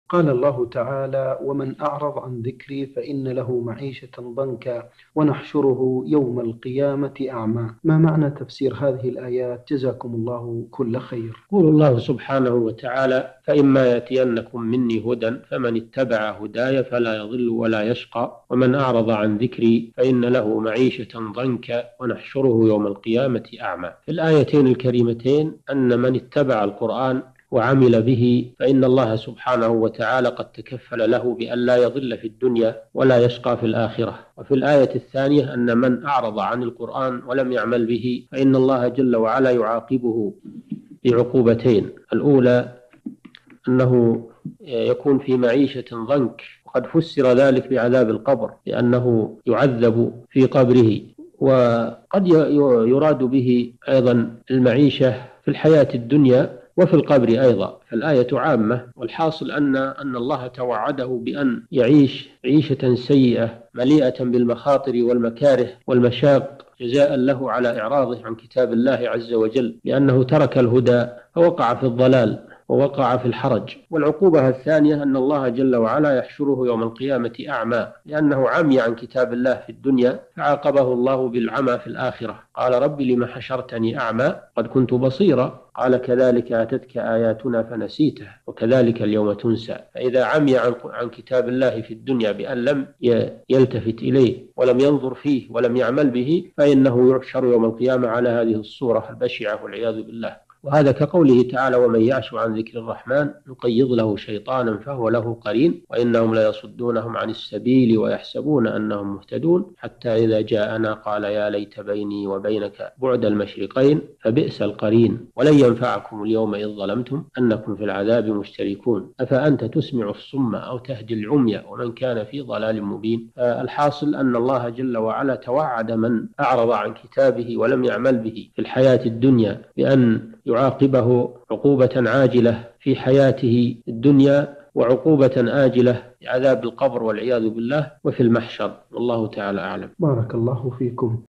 ما تفسير الآية (ومن أعرض عن ذكري فإن له معيشة ضنكا...)؟ الشيخ صالح بن فوزان الفوزان
نص السؤال:
قال الله تعالى (ومن أعرض عن ذكري فإن له معيشة ضنكا ونحشره يوم القيامة أعمى) ما معنى تفسير هذه الآية جزاكم الله كل خير؟

السيرة الذاتية :
للاشتراك في قناة الشيخ صالح الفوزان

رئيس الفريق
لمشروع كبار العلماء